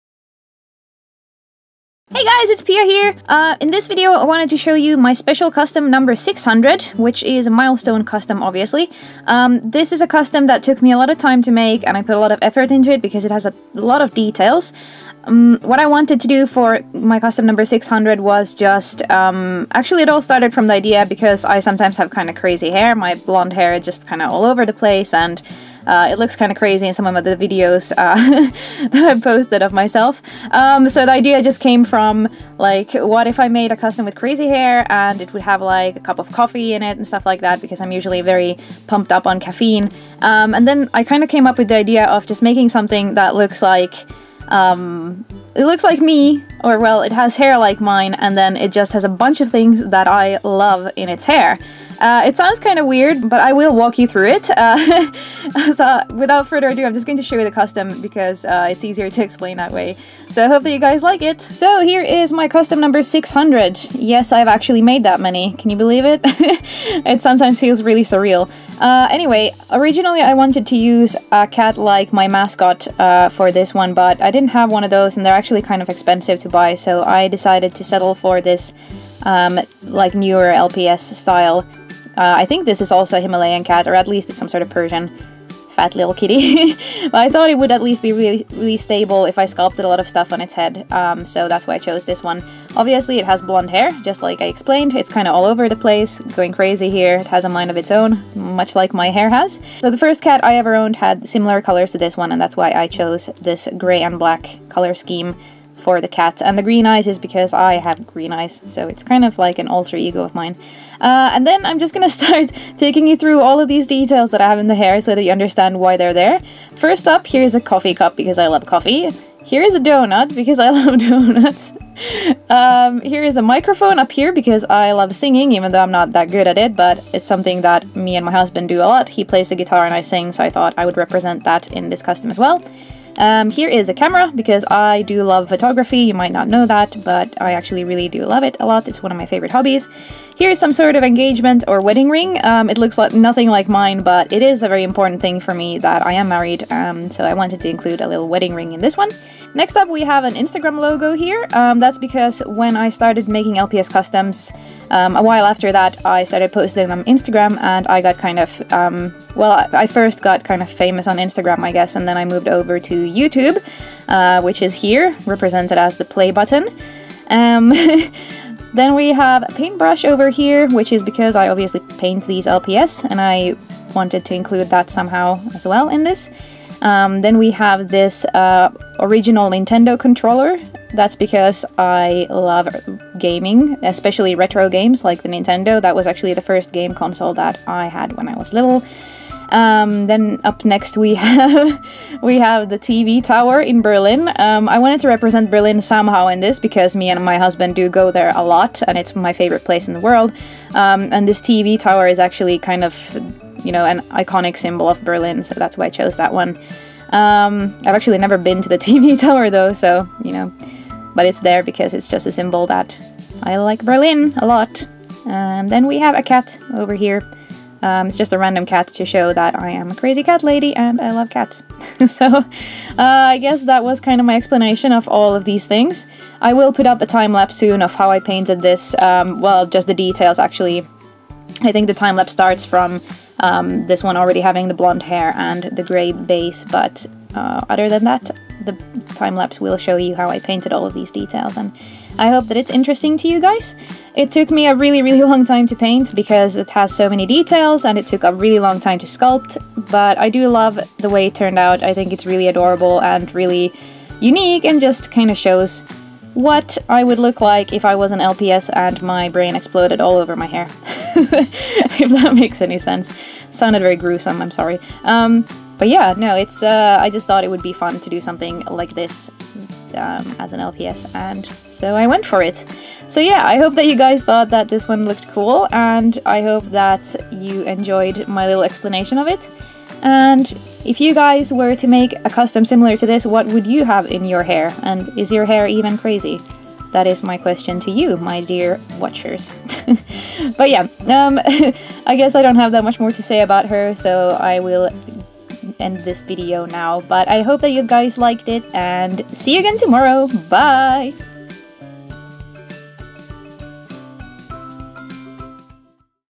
Special custom: Crazy hair kitty (#600)
Here's what I came up with for my special milestone custom #600! :) Hope you like my crazy haired kitty! :D

Littlest Pet Shop belong to Hasbro.